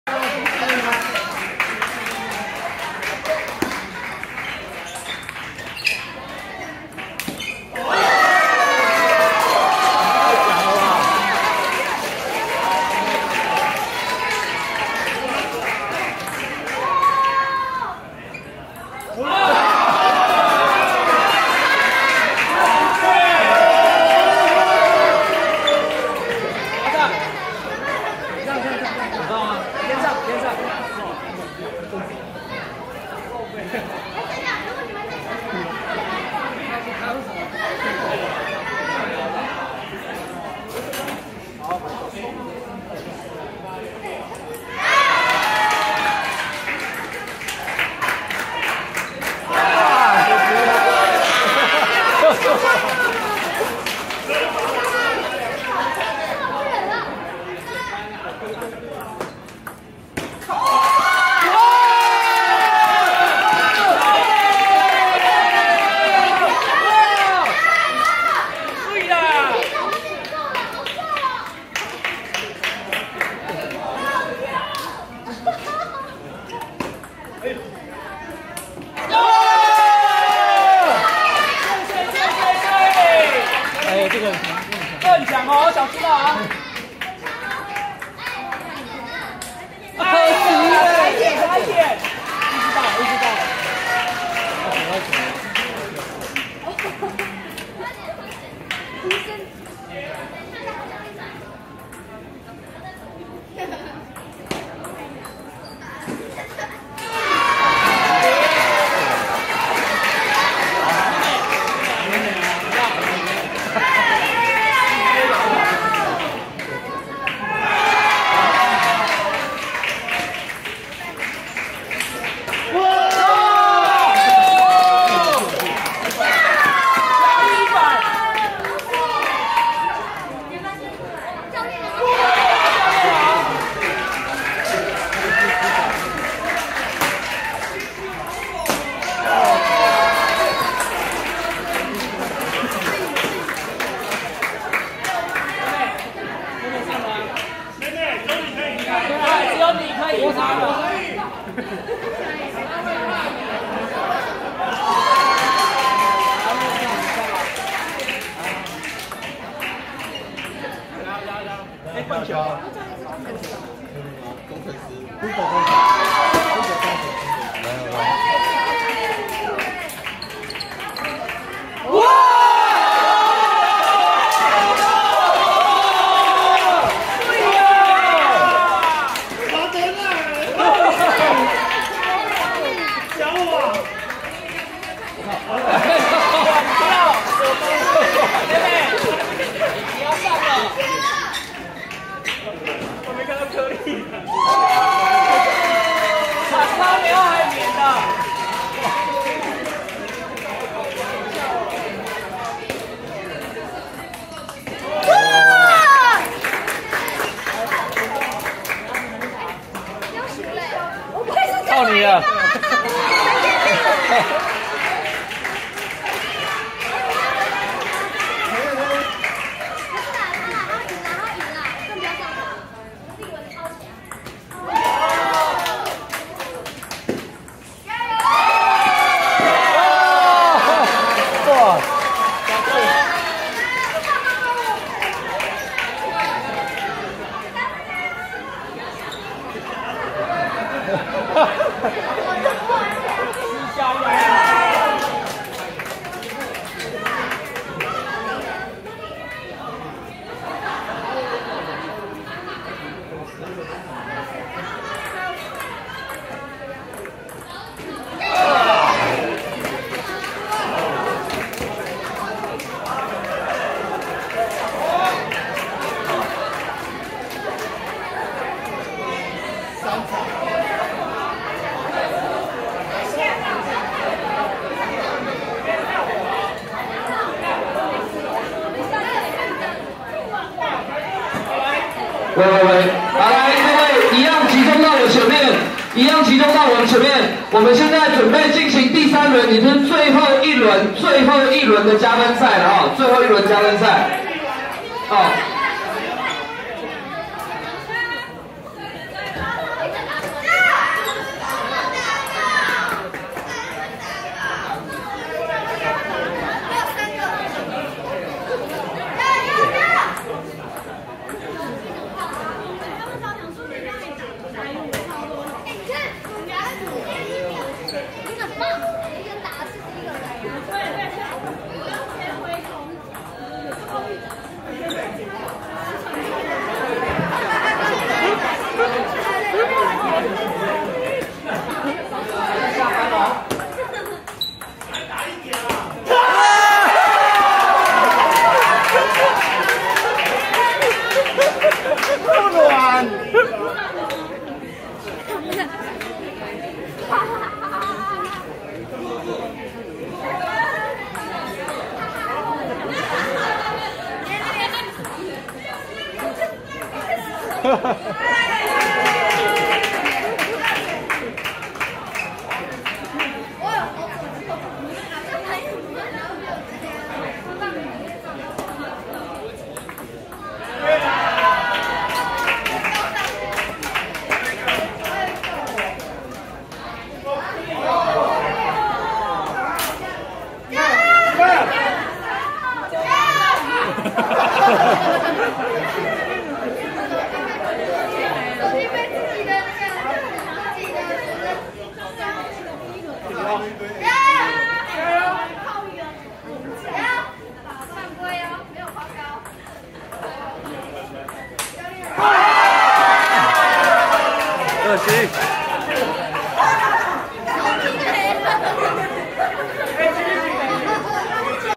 【全民桌訓·明星桌球運動會·一球定輸贏】車模女神Abby vs 氣質美人妻Amber 2023.02.25
本賽事由全民桌訓舉辦
全民打桌球·美少女之日
第三季明星桌球運動會
共有6位女神領軍參賽

「一分定輸贏之團隊殲滅賽」

規則：

1.兩隊各12名成員由隊長決定棒次，贏一個對手得一分，隊伍間先得到12分者勝。

2.贏一分的選手繼續留在場上、輸一分的選手則由下一棒接替。

3.由接替上場的選手發球，直至將對方整隊殲滅。

感謝神隊友澔緯支援影片